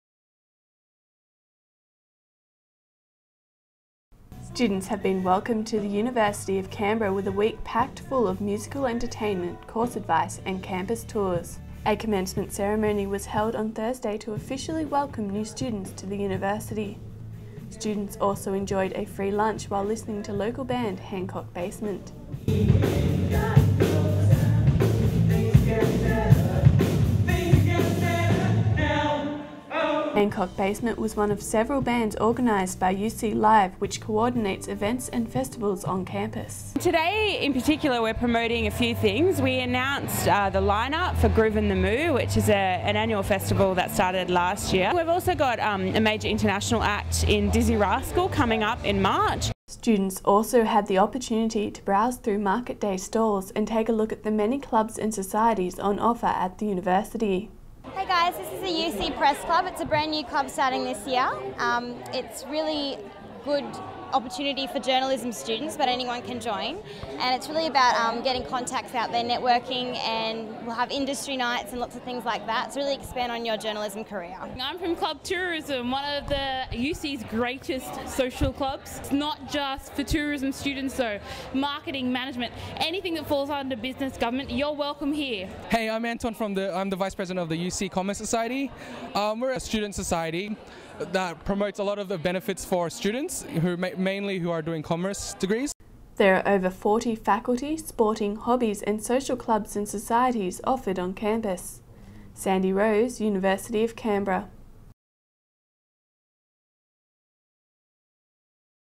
O-week 2011
University of Canberra has welcomed students with a week packed full of musical entertainment, course advice and campus tours.

Market day stalls were also a great way for students to find out what clubs and societies are on offer at the university.